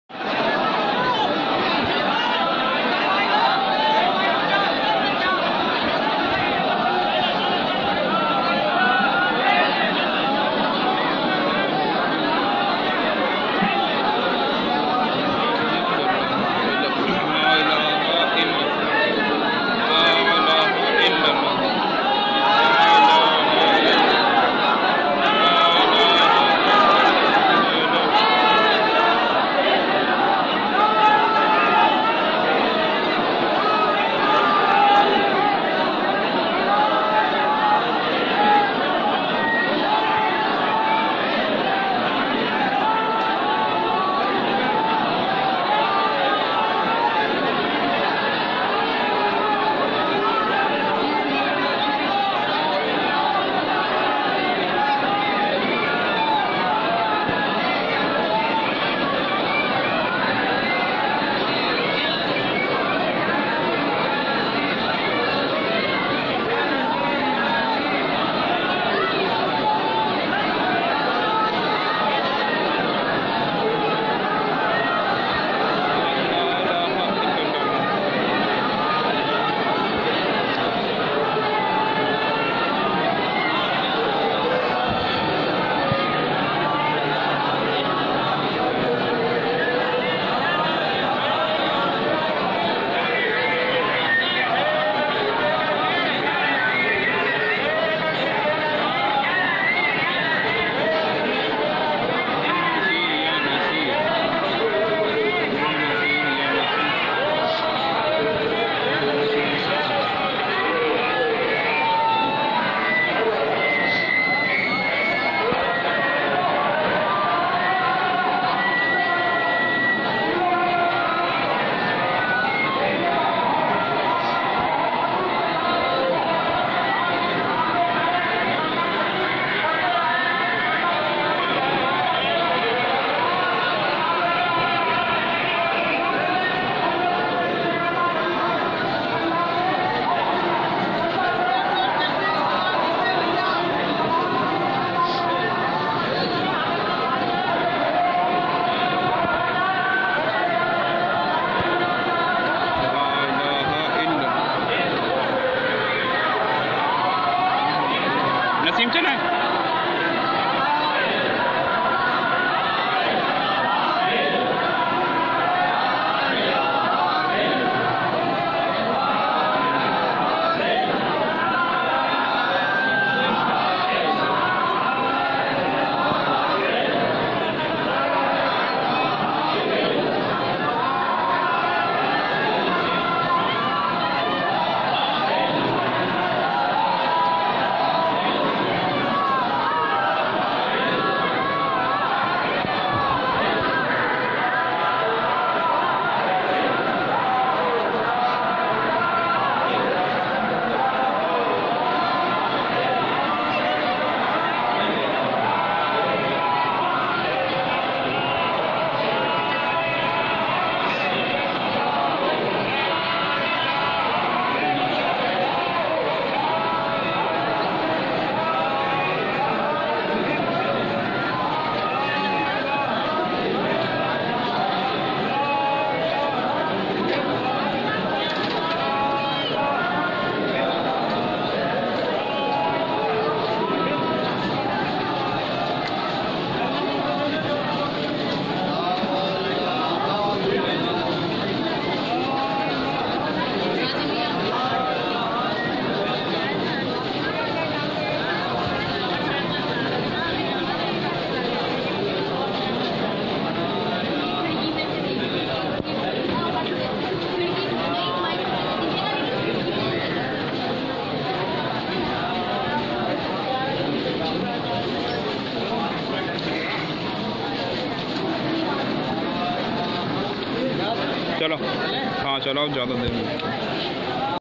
Huzoor Tajushshariya के जनाजे को लेजाते हुए | या अललाह मेरे ताजुशशरिया के दराजात को बलँद फरमा आमीन।
Ya Allah mere Tajushshariya k Darajaat ko  buland ata farmaaye.. Ameen

Or Tajushshariya k sadqe humsb musalmano ki Magfirat ata Farmaye.. Ameen

بستی بستی قریہ قریہ تاج الشریعہ تاج الشریعہ